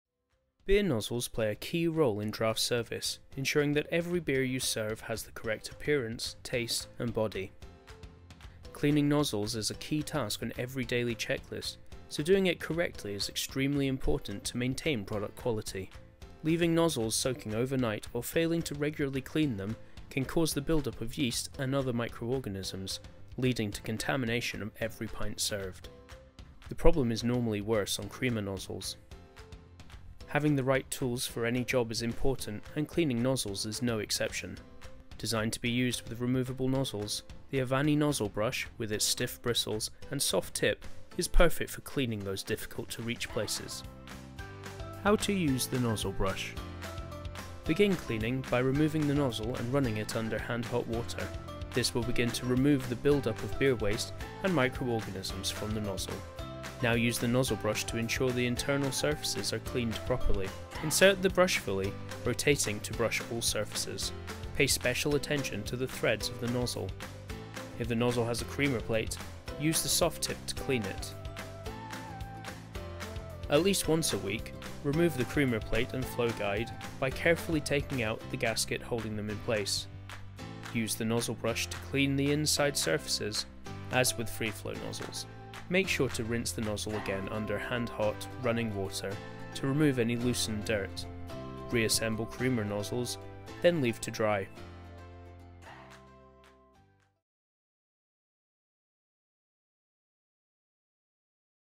How to clean beer nozzles with the Avani Nozzle Brush | Avani Solutions
Poor nozzle hygiene affects beer quality, contributing to off-flavours. Creamer plates are liable to clog up with dried beer and screw threads become sticky with beer residue, yeast, and bacteria.

Leaving nozzles to soak in water is not ‘cleaning’. That container of water becomes a huge petri dish of yeasts and bacteria, which leave a layer of slime on nozzles.

The simplest and most effective way to clean nozzles daily is to run them under clean warm water and brush away any residue from the inner surfaces using the Avani Nozzle Brush. The end of each brush has a soft, absorbent pad that can be used to clean creamer plates.

Buy Avani Nozzle Brushes (Pack of 10)
A bar essential - perfect for the daily cleaning of removable beer nozzles

Chapters

0:00 - Nozzle hygiene affects every pint poured
0:15 - Poor cleaning allows yeast and microorganisms to flourish!
0:35 - Introducing the Avani Nozzle Brush
0:44 - How to use the nozzle brush